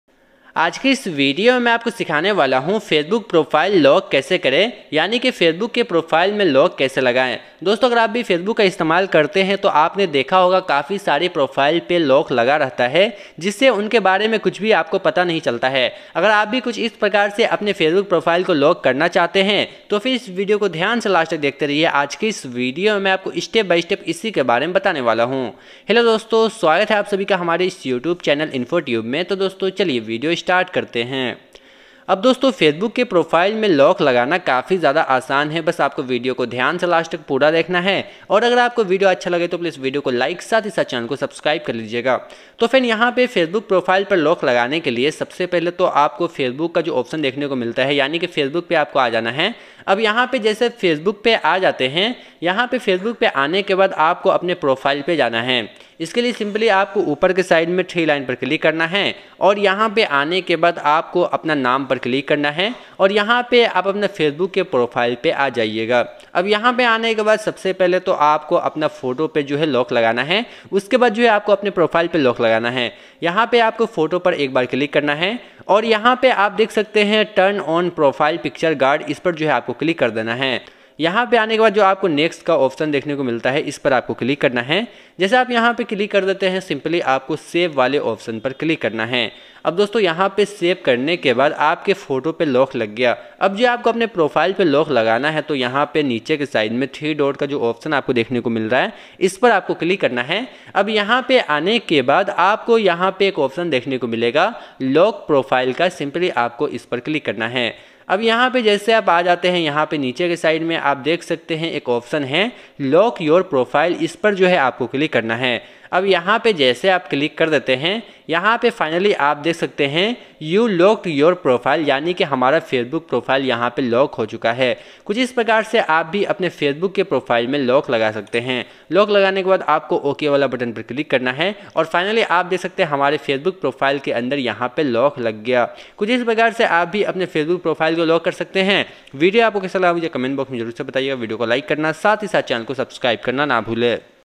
facebook profile lock kaise kare | facebook profile lock system | how to lock facebook profile
About :- dosto aajke video me apko batane wala hu facebook ka profile lock kaise kare ummed karta hu apko yeah video pasand aayegi